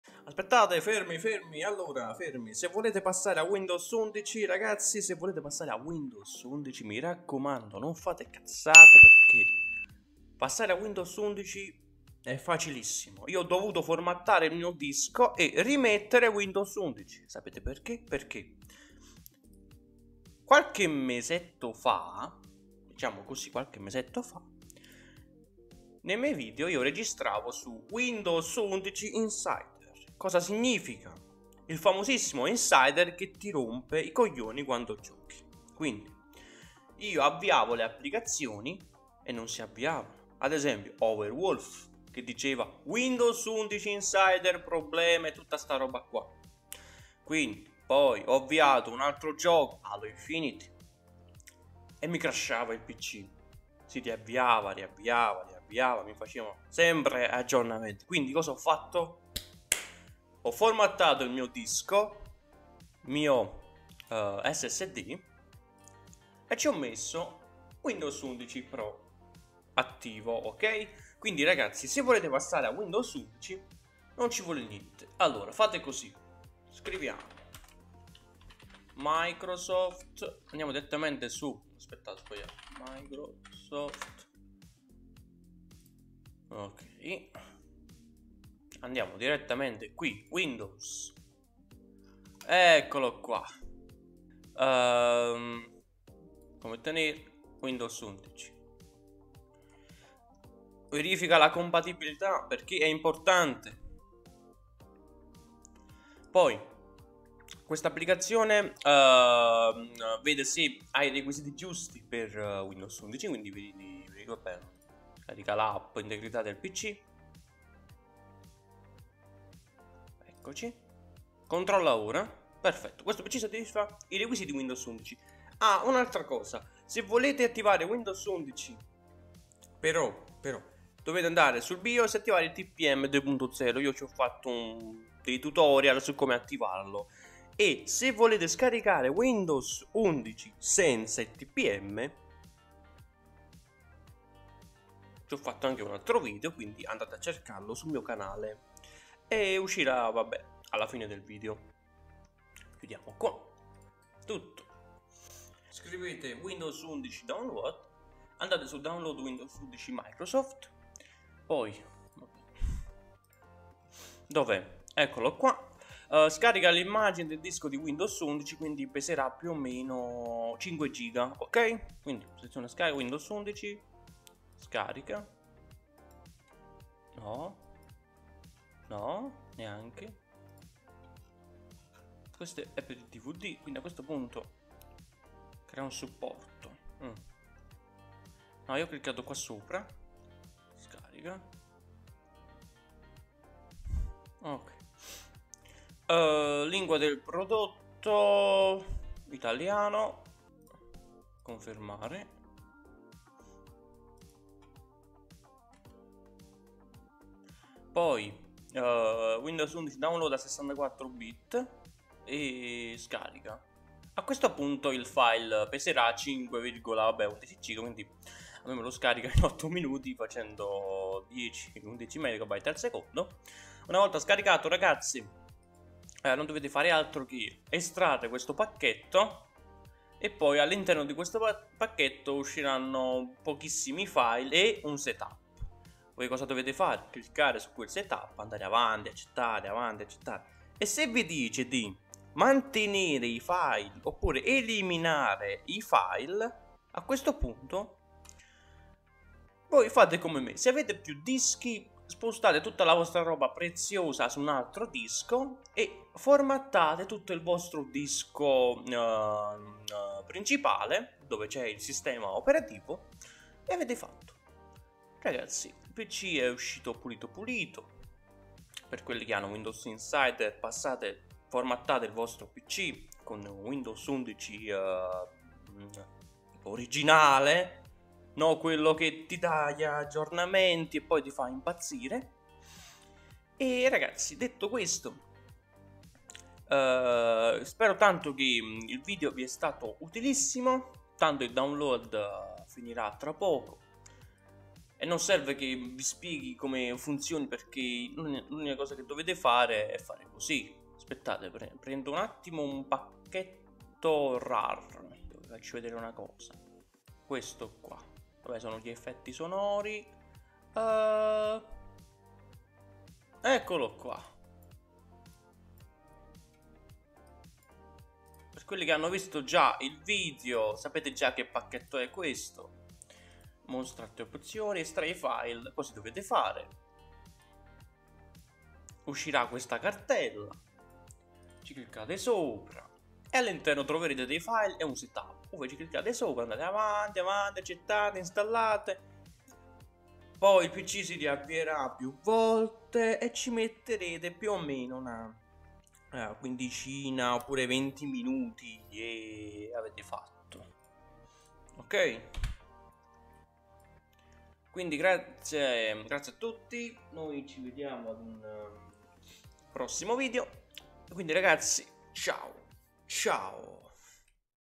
COME AGGIORNARE IL SISTEMA OPERATIVO A WINDOWS 11 E NON PASSATE A WINDOWS INSIDER (PERICOLOSO)
Bene gente è il momento di formattare tutto e reinstallare il nuovo windows 11 pro senza problemi

PC:
MOBO: B450 GAMING PLUS
CPU: R5 3600
GPU: 1660 SUPER
RAM: 16 GB
CASE: 4000D AIRFLOW CASE CORSAIR

VIDEO TUTORIAL
               GAMES---TECH---MICROSOFT